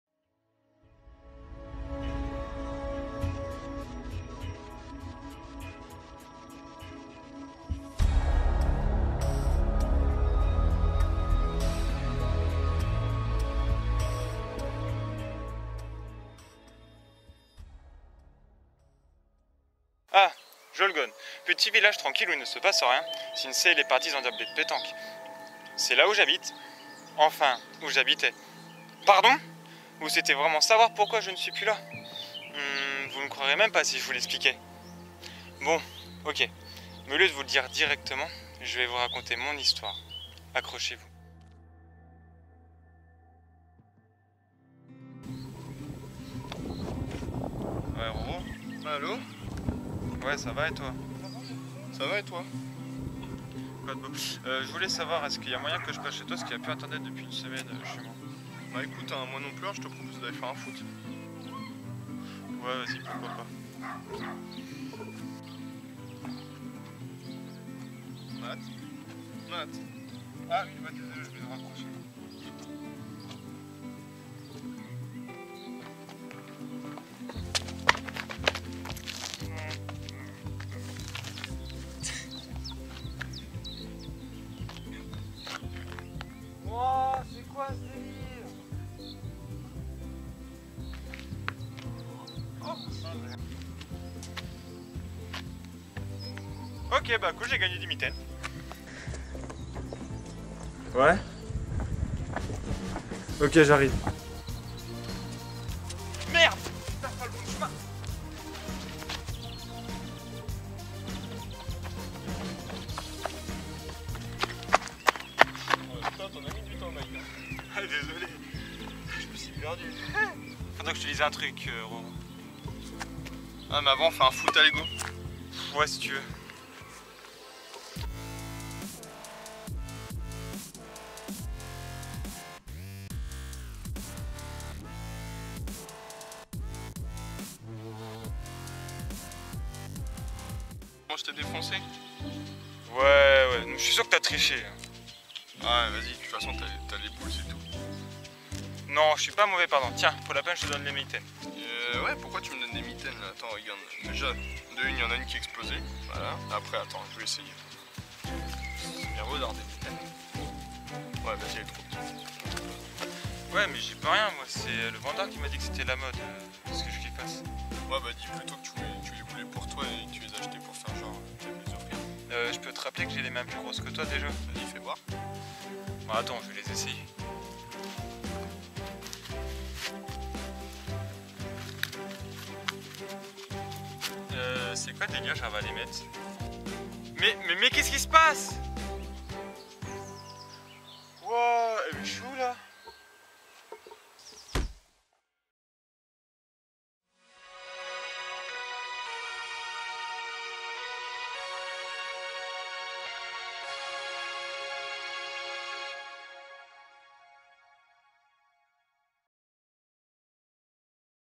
Valmora, web série : Coupure générale (Ep1 - saison 1) Chaine abandonné
Alors qu'une panne d'internet à lieu sur tout le village de Jaulgonne, Matt, jeune homme de 21 ans se voit obligé de changer ses habitudes et sortir jouer avec son ami, mais un élément perturbateur n'était pas prévu au programme.

Crann Cairdeas est un petit groupe amateur s'étant lancé dans la folle aventure que lui offre l'audiovisuel. Valmora est sa deuxième création et raconte, au travers d'une web série fantastique, l'histoire d'un jeune homme pour qui, coupure d'internet rime avec galère total.

Lien Utiles

Ne pas oublier